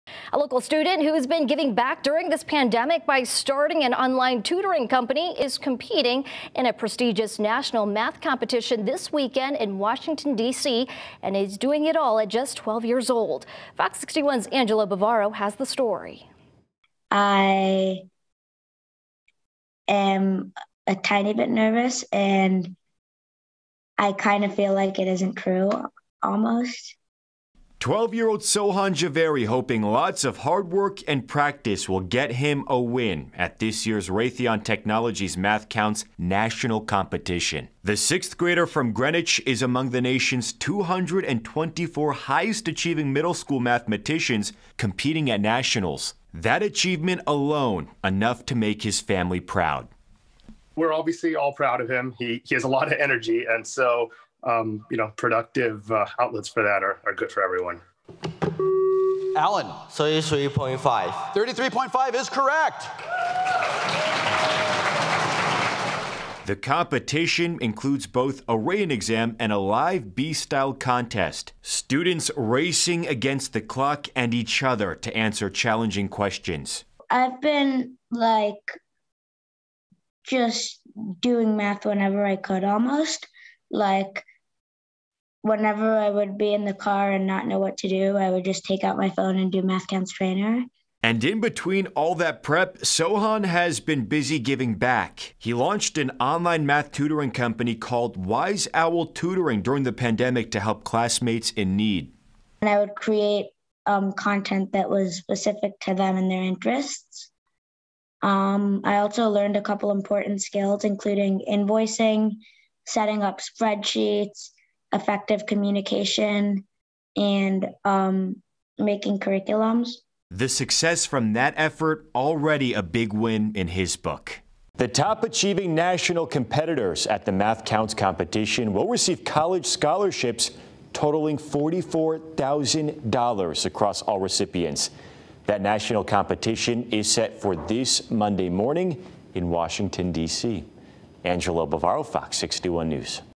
Greenwich 6th grader will put knowledge to the test at MATHCOUNTS competition
The 12-year-old has also launched an online math tutoring company called Wise Owl Tutoring.